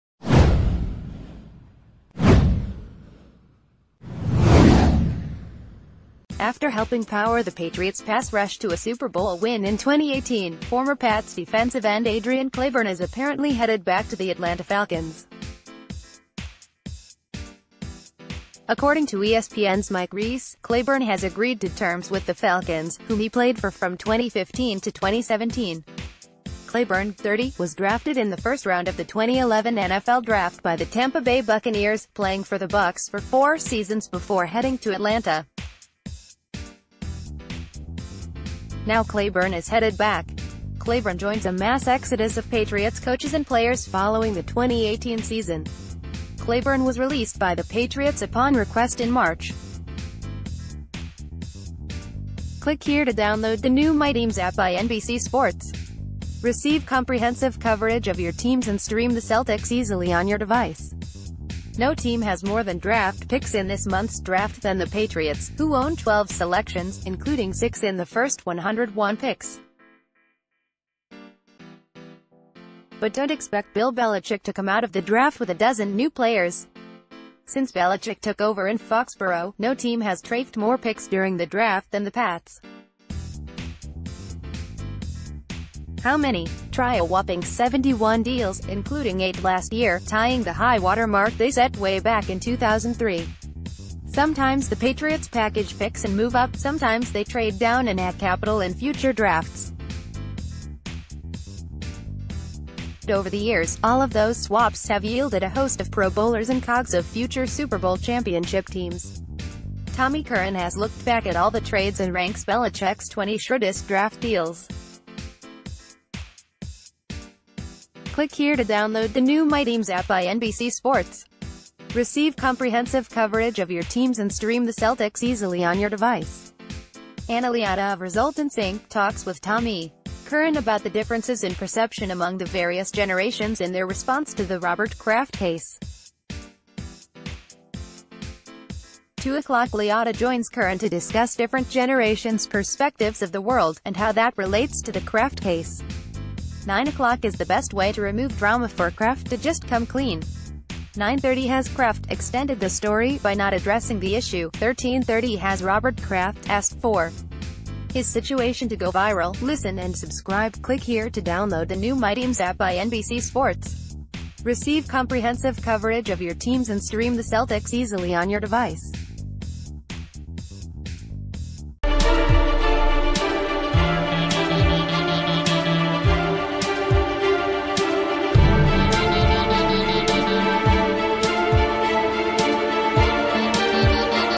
Former Pats DE Clayborn returns to Atlanta
After helping power the Patriots' pass rush to a Super Bowl win in 2018, former Pats defensive end Adrian Clayborn is apparently headed back to the Atlanta Falcons. According to ESPN's Mike Reiss, Clayborn has agreed to terms with the Falcons, whom he played for from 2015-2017. Clayborn, 30, was drafted in the first round of the 2011 NFL draft by the Tampa Bay Buccaneers, playing for the Bucs for four seasons before heading to Atlanta. Now Clayborn is headed back. Clayborn joins a mass exodus of...